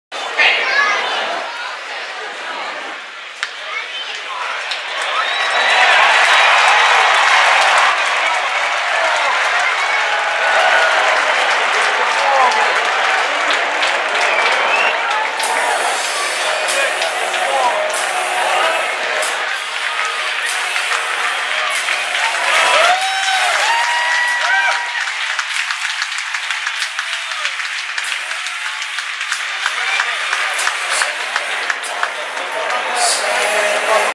Eric Campbell's First Major League Hit
Mets first basemen Eric Campbell hit his first Major League base hit in the bottom of the 4th inning off of The Phillies pitcher Cole Hamels on May 11th, 2014